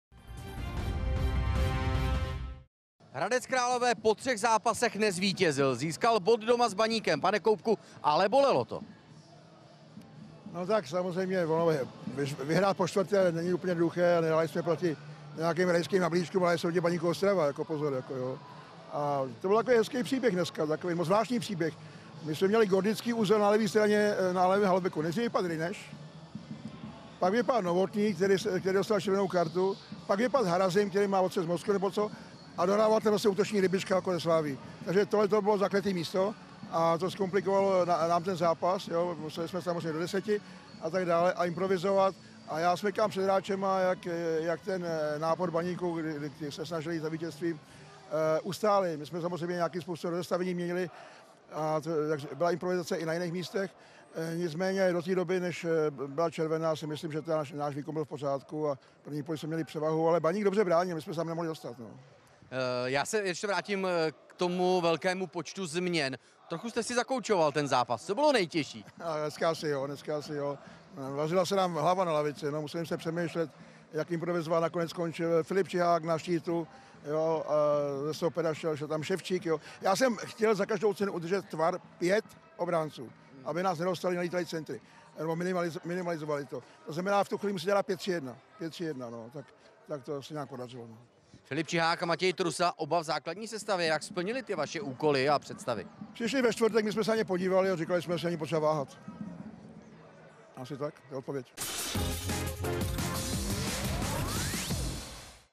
Koubek po náročném duelu s Baníkem: Vařila se nám na lavičce hlava
Hradec Králové po třech výhrách v řadě remizoval s Baníkem bez branek. Miroslav Koubek byl ale s bodem spokojený, jednak jeho tým dohrával v deseti, jednak musel složitě vymýšlet složení sestavy. „Vařila se nám na lavičce hlava, museli jsme improvizovat, navíc jsme měli gordický uzel na levém halvbeku,“ pravil zkušený kouč. Jak se mu líbily nové posily?